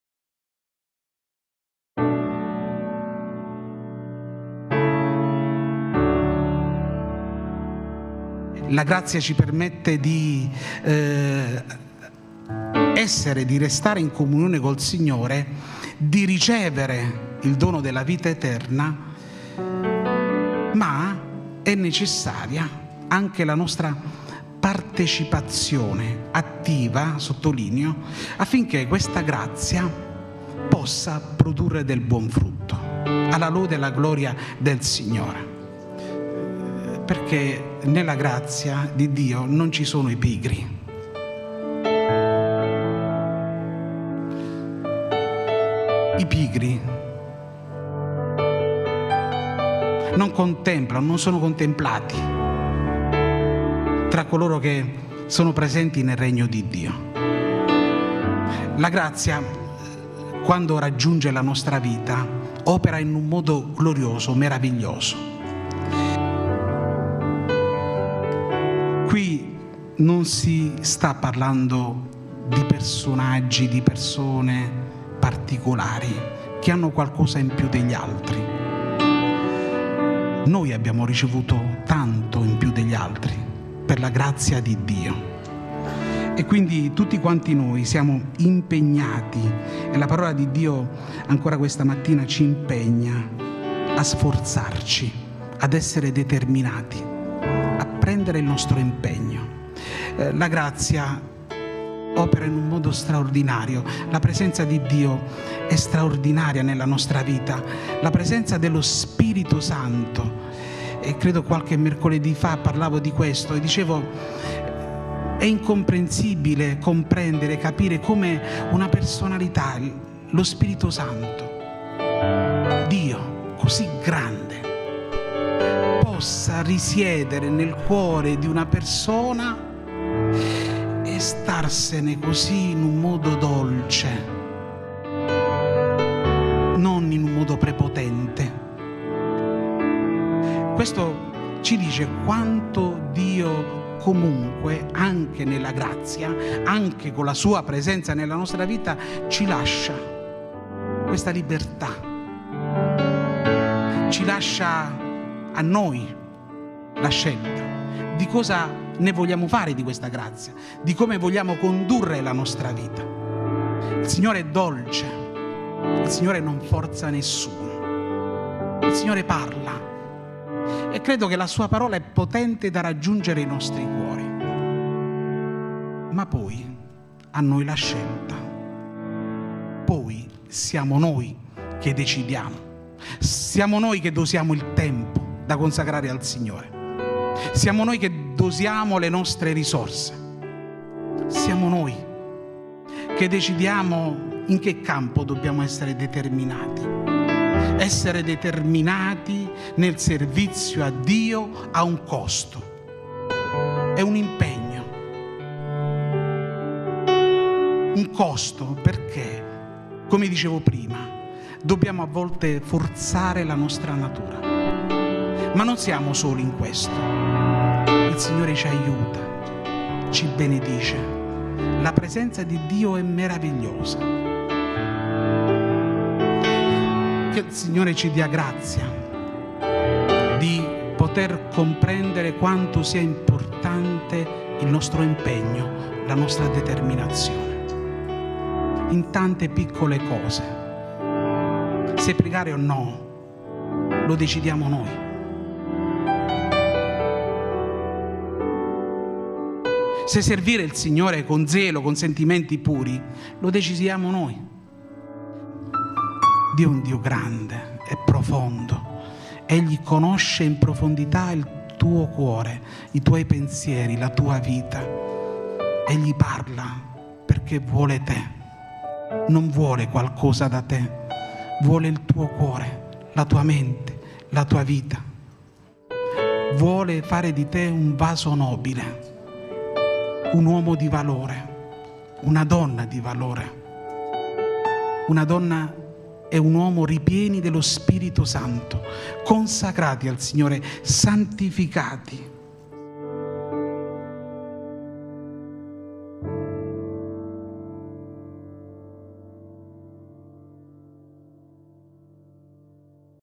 "Servitori approvati" - Brevi Riflessioni - Daniele Ramunno
La grazia di Dio ci permette di restare in comunione con Lui e di ricevere il dono della vita eterna.

È necessaria però anche la nostra partecipazione attiva, affinché questa possa produrre del buon frutto per la gloria di Cristo.

Nella grazia di Dio non ci sono i pigri, perché non sono presenti nel regno di Dio.

La grazia di Dio dal momento in cui raggiunge la nostra vita, opera in un modo glorioso, unico e meraviglioso.

Noi che abbiamo ricevuto il privilegio di ricevere la grazia di Dio non siamo persone particolari, o meglio di altre, ma ogni cosa che riceviamo da Lui è un dono che non abbiamo meritato, ma che ci è stato offerto da Gesù.

Quindi tutti quanti noi siamo impegnati e la parola di Dio ci impegna a sforzarci, ad essere determinati, a prendere parte al nostro impegno.

La grazia opera in un modo straordinario, la presenza di Dio è straordinaria nella nostra vita, la presenza dello Spirito Santo è straordinaria e fondamentale per noi.

Sapete, è difficile comprendere come Dio, così grande ed eccelso, possa risiedere nel cuore di una persona immeritevole e nonostante questo albergare nelle vite in un modo così dolce e amorevole senza prepotenza.

Questo ci dice tanto su quanto Dio ci lasci liberi di scegliere su cosa farne di questa grazia, di come vogliamo condurre la nostra vita.

Il Signore è dolce, non forza nessuno.

È il Signore che parla attraverso la Sua parola che è potente da raggiungere i nostri cuori, ma poi a noi la scelta, siamo noi che decidiamo, siamo noi che dosiamo il tempo da dare al Signore, siamo noi che dosiamo le nostre risorse, siamo noi che decidiamo in che campo dobbiamo essere determinati.

Essere determinati nel servizio a Dio è un grande impegno che richiede un costo, in quanto a volte dobbiamo forzare e andare contro la nostra natura umana.

Ma in tutto questo il Signore ci aiuta, ci benedice.

La presenza di Dio è meravigliosa!

Che Signore ci dia grazia di poter comprendere quanto sia importante il nostro impegno e la nostra determinazione nelle grandi e nelle piccole cose.

Decidiamo noi se essere pigri oppure se servire il Signore con Zelo e con sentimenti puri.

Abbiamo un Dio grande e profondo che conosce in profondità il nostro cuore, i nostri pensieri, la nostra vita e che parla perché vuole il tuo cuore, la tua mente, la tua vita, vuole fare di te un vaso nobile, un uomo di valore, una donna di valore, una donna e un uomo ripieni dello spirito santo, consacrati al Signore e santificati.